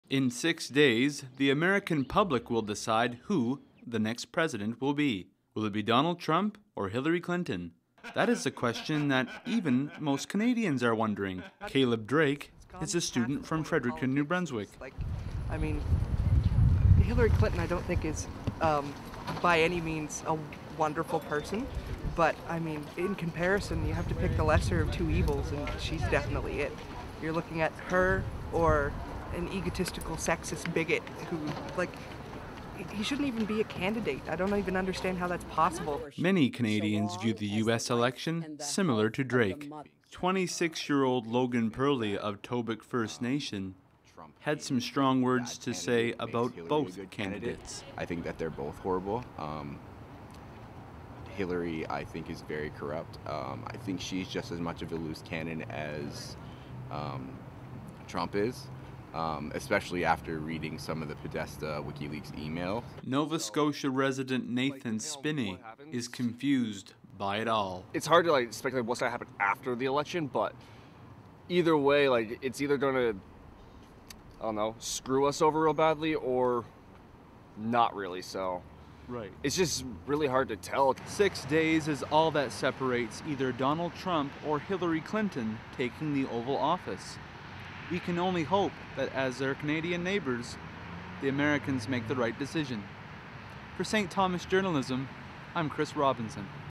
Canadian Perspective on the US Election
There's never been an election covered as closely as the current one between Trump and Clinton. As a Canadian, I wanted to find the Canadian perspective of what my fellow Canadians think of the American' election.

Listen to three Maritimers give their opinion on the election race.

I'm a fourth year journalism student with a minor in gerontology at St. Thomas University in Fredericton,NB, Canada. I made this for our bi-weekly show "The Basement Files."